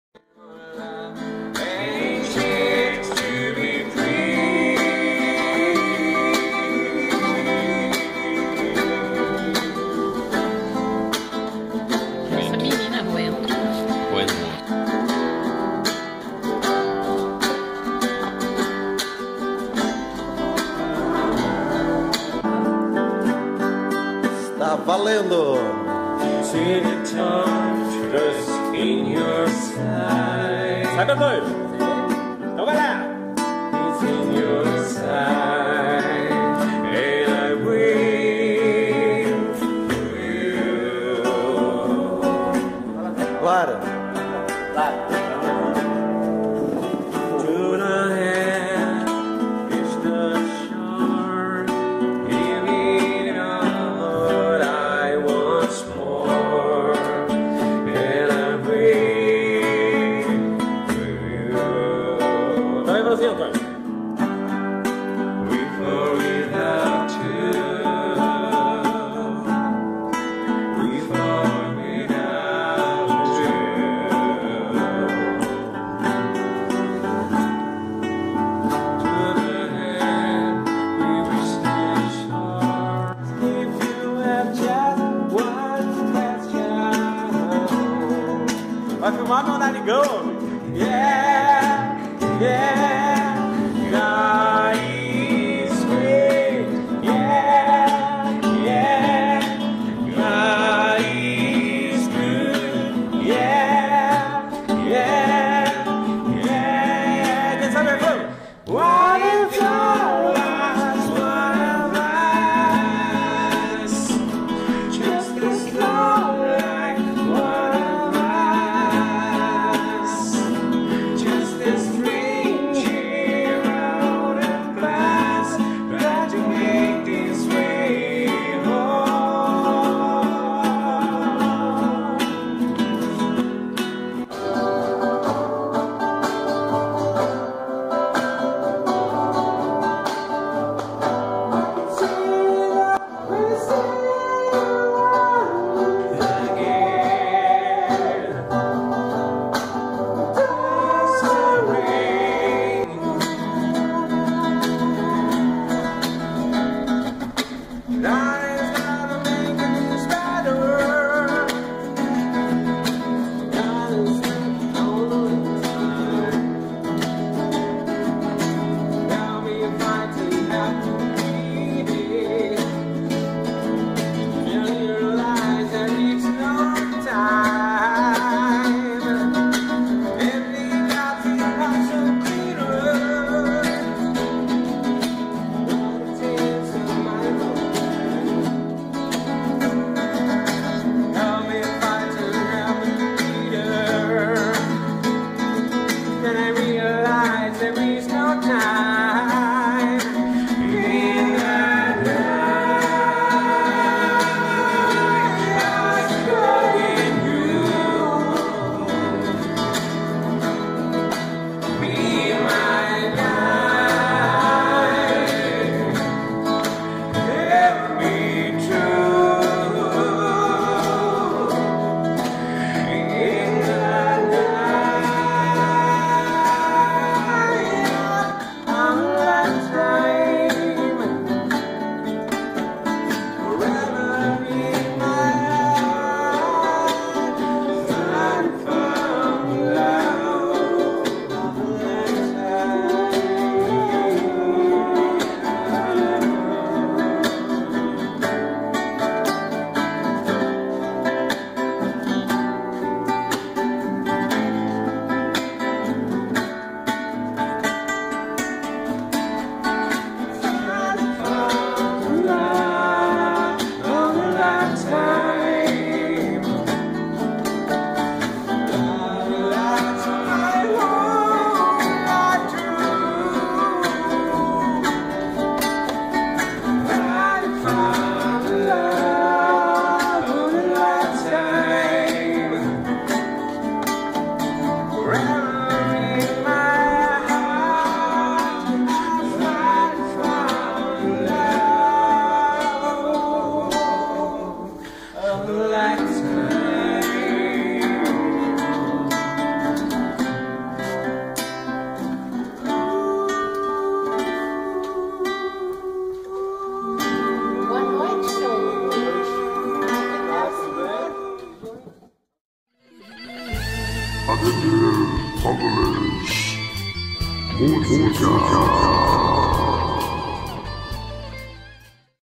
ADEMIR HOMRICH & Friends - Sarau Na Casa da Mãe (Helloween, Firehouse, etc) - #152
Na noite de 11/08/2016 fizemos um pequeno SARAU na Casa da Mãe. Foi muito legal tocar as músicas que mais gostamos e estar com a Família e alguns amigos.
Os "melhores momentos" dessa noite de Música que durou até a 1h da manhã estão registrados nesse vídeo. Espero que apreciem!
Um grande abraço e muito sucesso para todos nós!